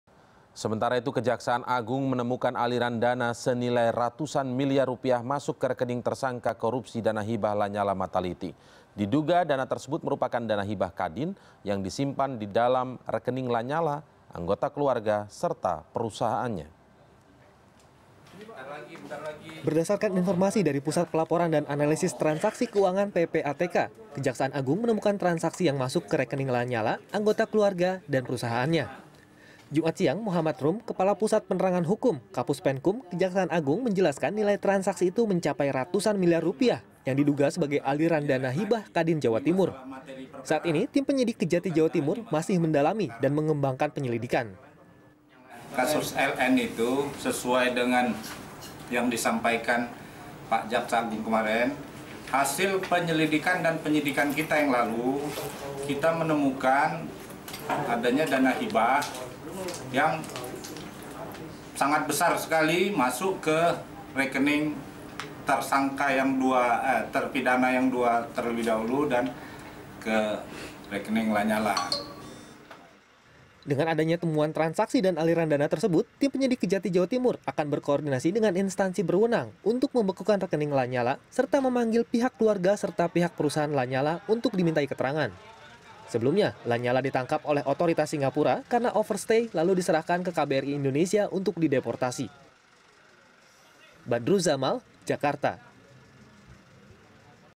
Penyidikan Aliran Dana Hibah ke Keluarga La Nyalla
Kejaksaan Agung menemukan aliran dana senilai ratusan miliar rupiah masujk ke rekening tersangka korupsi dana hibah, La Nyalla Mattalitti. Diduga dana tersebut merupakan dana hibah Kadin yang disimpan didalam rekening La Nyalla, anggota keluarga serta perusahaanya.